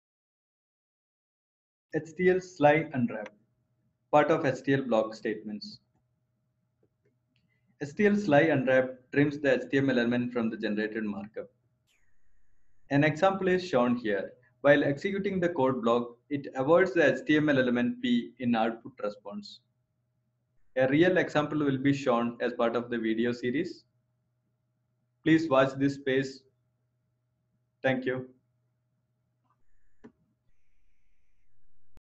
12 HTL Sly Unwrap
This video is part of HTL Tutorial Series and gives an overview on HTL Sly Unwrap and usage.